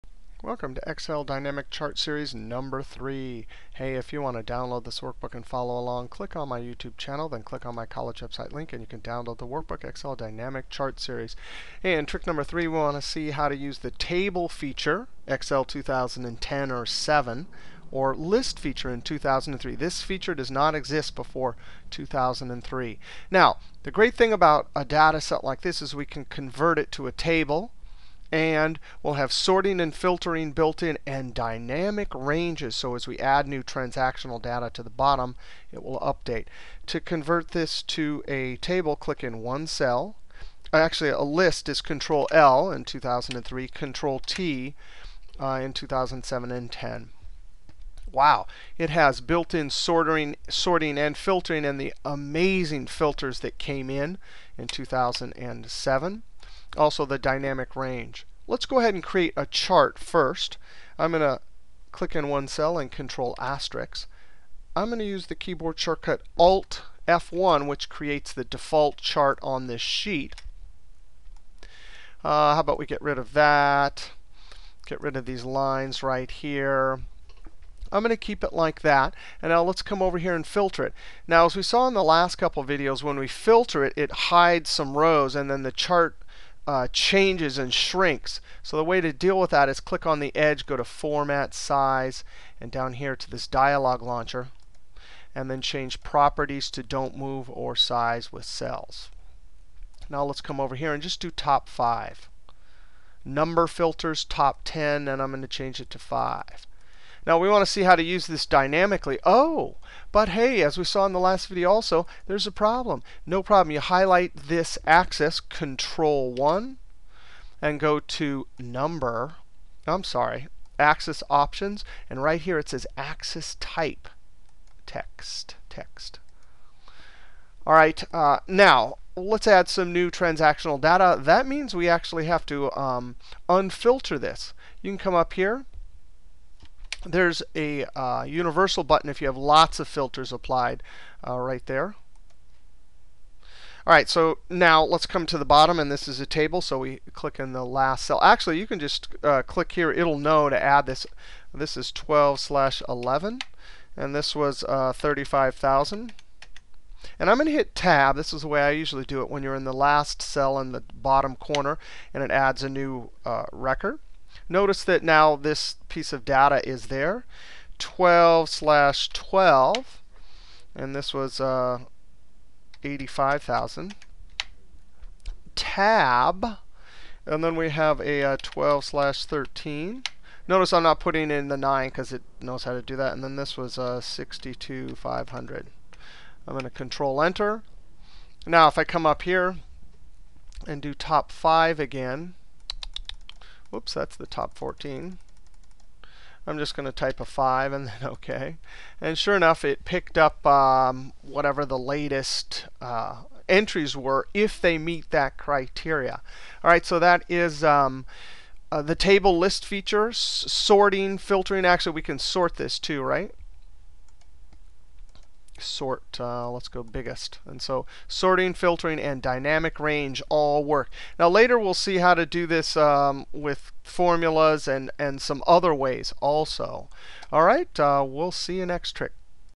Excel Dynamic Chart #3: Table Feature (Excel 2010 / 2007) List Feature (Excel 2003)
THE BEST dynamic chart trick around: If you setup data with the Excel 2010 / 2007 Table Feature (2003 List Feature) these things are possible:
1)Sort change chart
2)Unbelievable filtering options in Excel 2007 / 2010
3)Dynamic ranges for data added to the bottom of the table
4)And the most amazing aspect is that Sort, Filter and dynamic ranges all work together

Chart created from transactional data  not aggregated data.